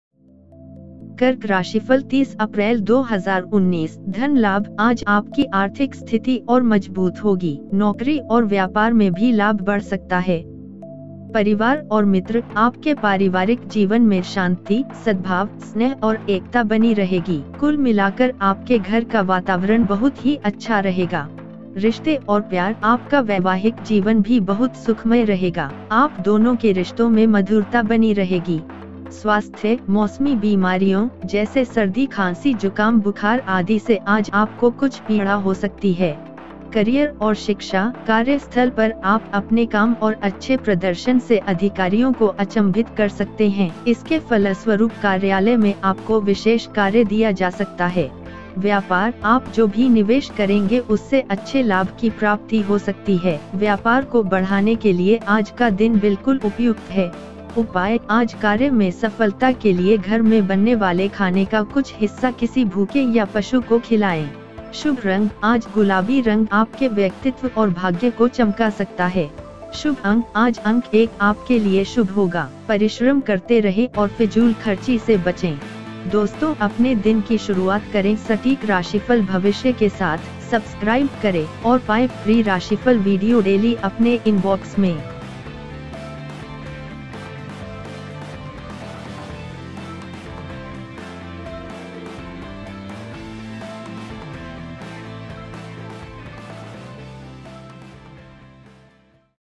30 April 2019 कर्क राशि | आज का राशिफल | Daily Rashifal | Dainik Rashifal | today horoscope
वीडियो में दी गयी जानकारी इंटरनेट पर के आधार पर दी गयी है
Bhagyashali Totke चैनल इसके सच या झूठ होने का दावा नहीं करता.

परिवार और मित्र  :- आपके पारिवारिक जीवन में शांति, सद्भाव, स्नेह और एकता बनी रहेगी। कुल मिलाकर आपके घर का वातावरण बहुत ही अच्छा रहेगा।

रिश्ते और प्यार  :- आपका वैवाहिक जीवन भी बहुत सुखमय रहेगा। आप दोनों के रिश्तों में मधुरता बनी रहेगी।

स्वास्थ्य :- मौसमी बीमारियों, जैसे- सर्दी खांसी जुकाम बुखार आदि से आज आपको कुछ पीड़ा हो सकती है।

करियर और शिक्षा: कार्य स्थल पर आप अपने काम और अच्छे प्रदर्शन से अधिकारियों को अचंभित कर सकतें हैं। इसके फलस्वरुप कार्यालय में आपको विशेष कार्य दिया जा सकता है

Family and Friends: - Peace, harmony, affection and unity will remain in your family life. Overall, your home environment will be very good.

Relationships and Love: - Your marital life will also be very pleasant. There will be sweetness in your relationship.

Health: - You may have some pain today from seasonal diseases such as cold cough cold fever.

Career and education: At the work place you can surprise the officials with their work and good performance. As a result, you can be given special work in the office.